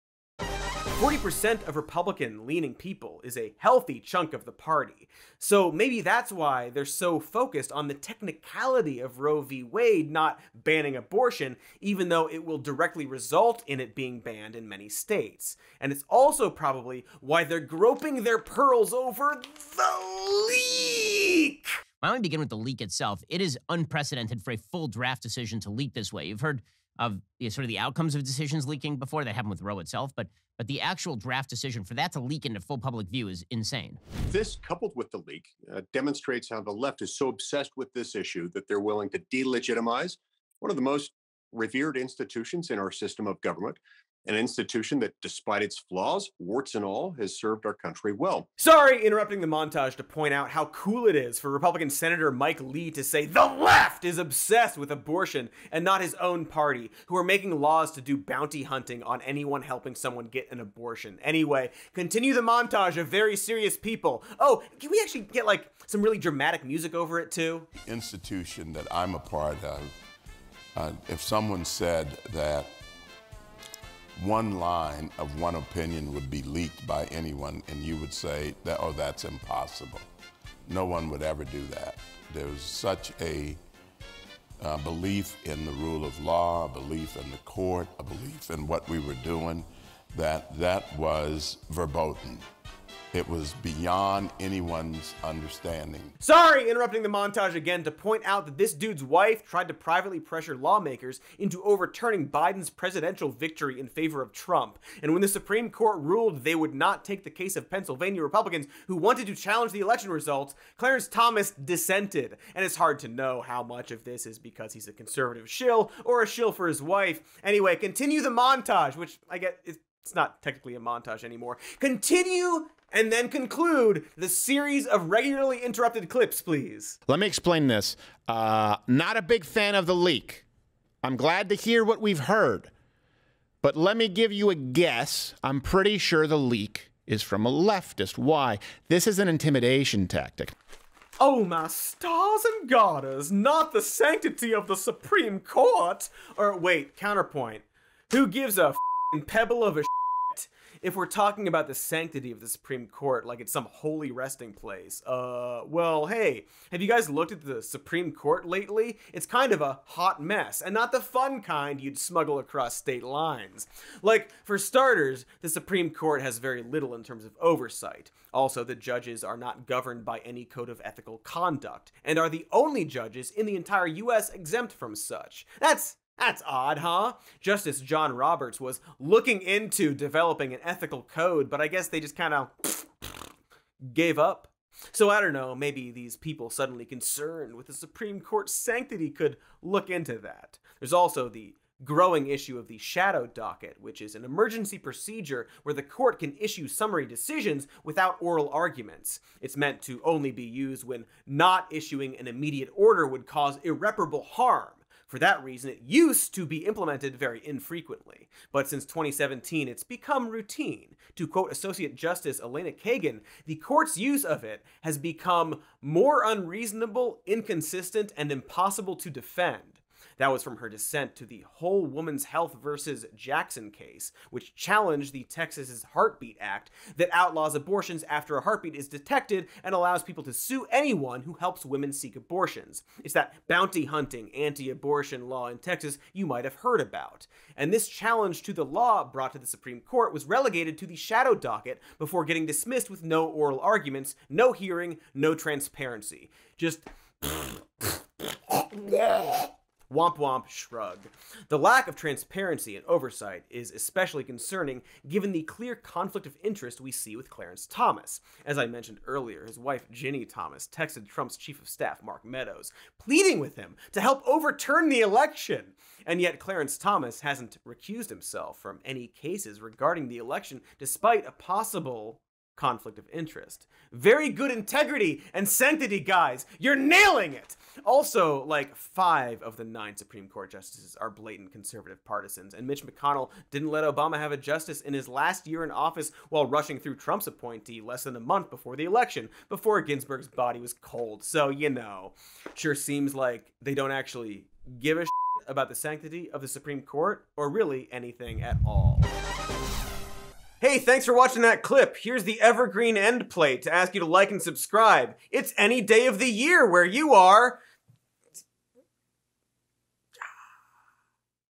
The Dum-Dums Who Want To Convince You Overturning Roe V Wade Is Actually Good
Hi. Wait, Steven Crowder and Ben Shapiro are wrong? Who'd a thunk it.

Executive Producer - Katy Stoll (@KatyStoll). Writer - Katie Goldin (@KatieGoldin). Directed by Will Gordh (@will_gordh). Edited by John Conway. Graphics by F. Clint DeNisco. Head Writer - David Christopher Bell. Producer - Nick Mundy. Researcher - Marco Siler-Gonzales (@mijo_marco). Associate Producer - Quincy Tucker (@quincyrashard).